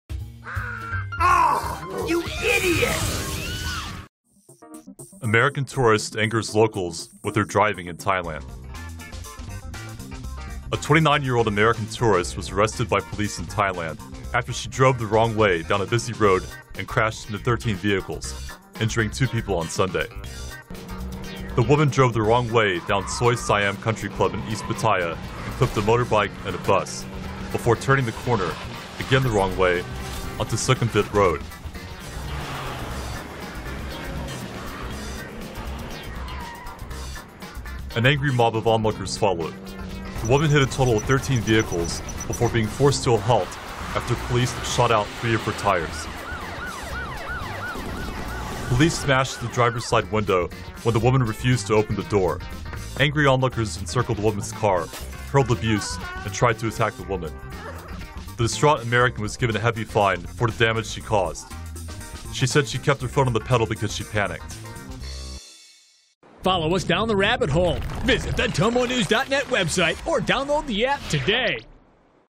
Crazy driving: Confused American tourist driving the wrong way crashes into 13 vehicles in Thailand
A 29-year-old American tourist was arrested by police in Thailand after she drove the wrong way down a busy road and crashed into 13 vehicles, injuring two people on Sunday.

The rampage began when the woman driving a white Toyota Innova, drove the wrong way down Soi Siam Country Club in East Pattaya and clipped a motorbike, injuring the female motorist.

The panicked driver did not stop and carried on driving. The Toyota then hit a bus before turning the corner, again the wrong way, onto Sukhumvit Road.

The woman hit a total of 13 vehicles before being forced to halt after police shot out three of her tires. Police smashed the driver's side window when the woman refused to open the door.

Angry onlookers, including the female motorist that she hit earlier, encircled the woman's car, hurled abuse and tried to attack the woman.

The distraught woman was given a hefty fine for the damage she caused. She said she kept her foot on the pedal because she panicked.

''It was a crazy bit of driving,'' one police officer said. ''The American woman struck a motorcycle going the wrong side down the road then kept going. She turned a corner and kept hitting other vehicles, 13 at last count.''

The woman may have been confused as Thais drive on the left side of the road while Americans drive on the right side of the road.